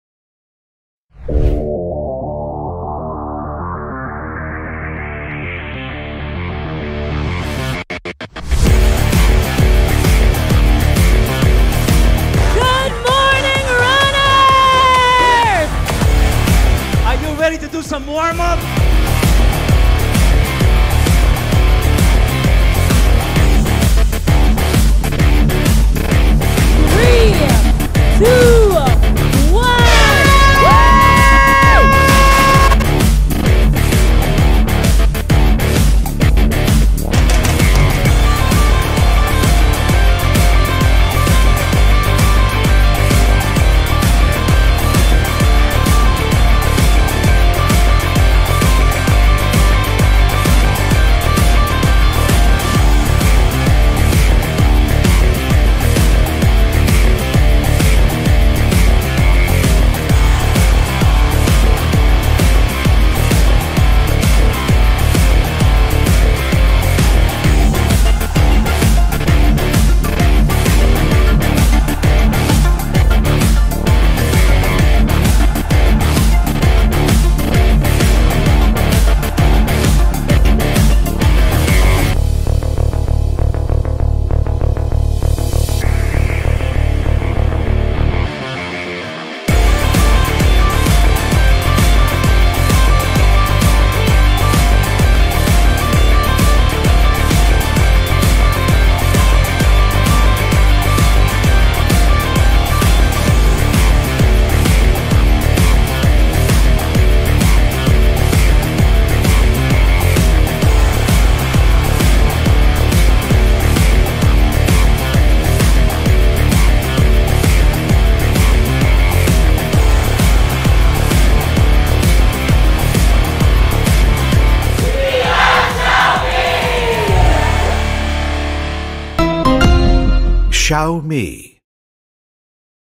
Xiaomi Pop Run 2023 | Philippines
Experience the thrill all over again! Relive the energy, the enthusiasm, and the extraordinary moments during the XiaomiPOPRun2023 XiaomiPhilippines🏃‍♂️🏃‍♀️🏃